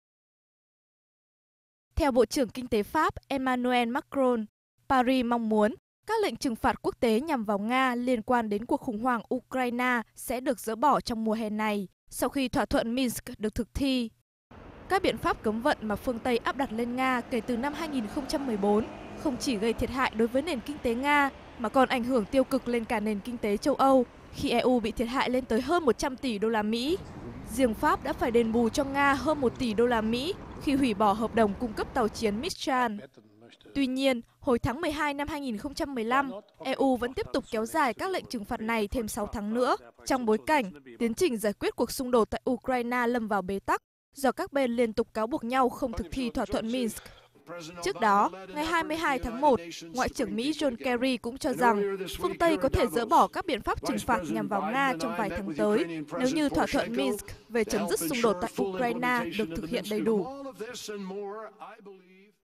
Pháp thúc đẩy việc dỡ bỏ lệnh trừng phạt Nga trong năm nay
Theo Bộ trưởng Kinh tế Pháp Emmanuel Macron, Paris mong muốn các lệnh trừng phạt quốc tế nhằm vào Nga liên quan đến cuộc khủng hoảng Ukraine sẽ được dỡ bỏ trong mùa ​Hè này, sau khi thỏa thuận Minsk được thực thi.

Các biện pháp cấm vận mà phương Tây áp đặt lên Nga kể từ năm 2014 không chỉ gây thiệt hại đối với nền kinh tế Nga mà còn ảnh hưởng tiêu cực lên cả nền kinh tế châu Âu khi EU bị thiệt hại lên đến hơn 100 tỷ USD./.